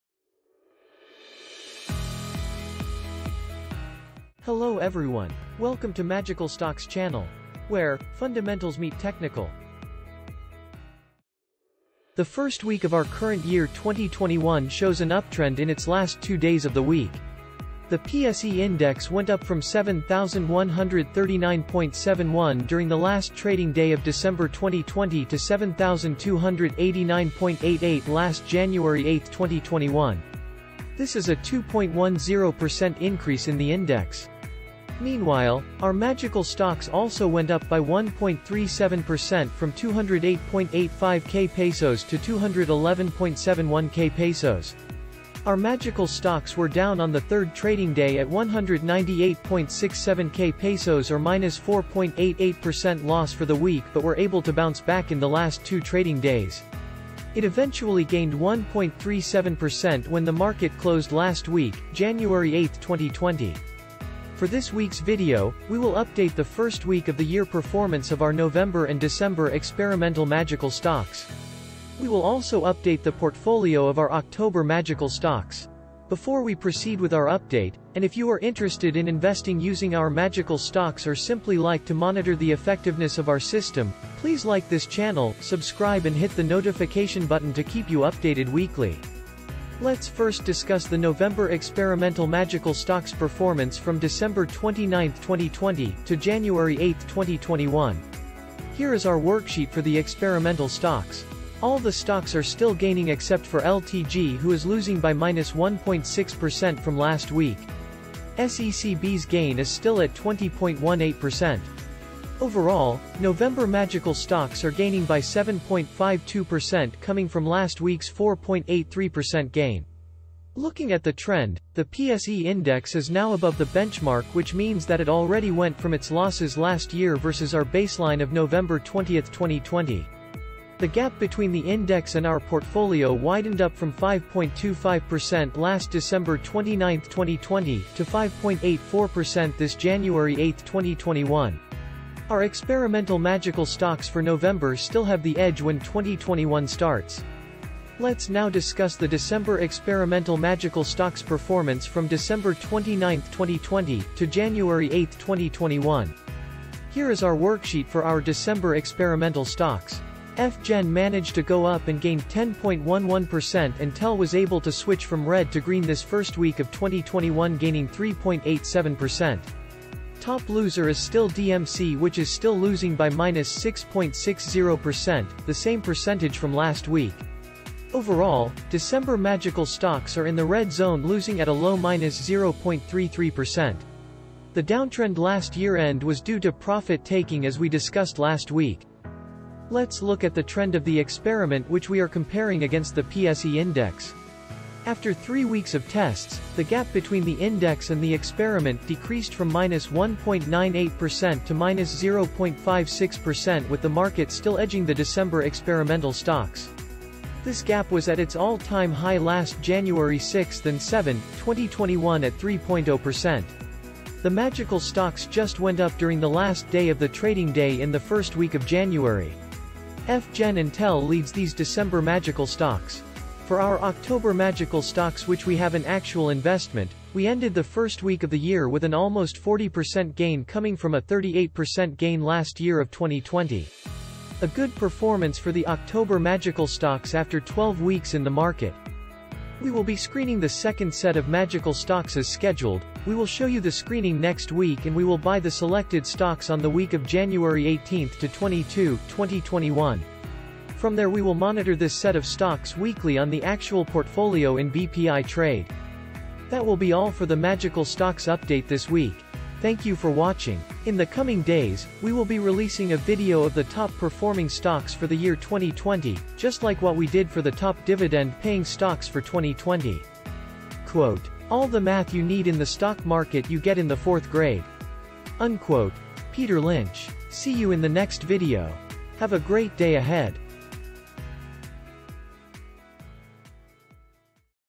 FIRST WEEK OF 2021: PORTFOLIO UPDATE
In this video, we will be showing an update of our portfolio for the 1st week of 2021.